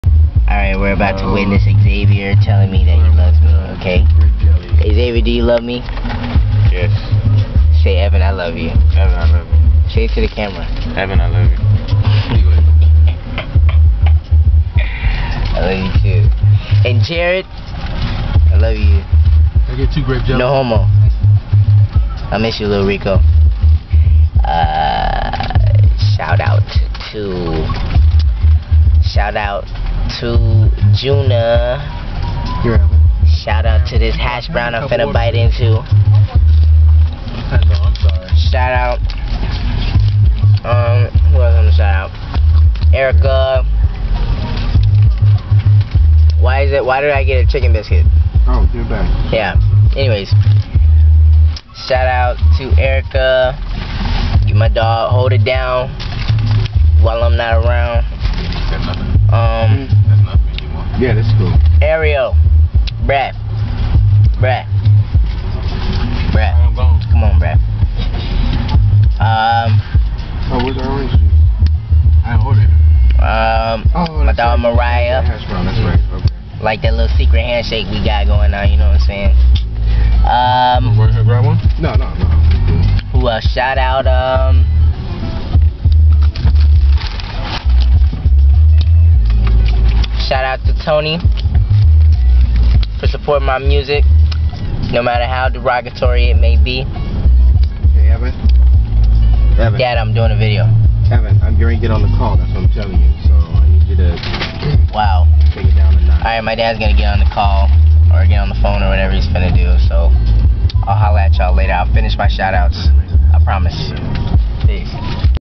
Shout Outs...random
me on tha way to my college shoutin out my niggas n bitches...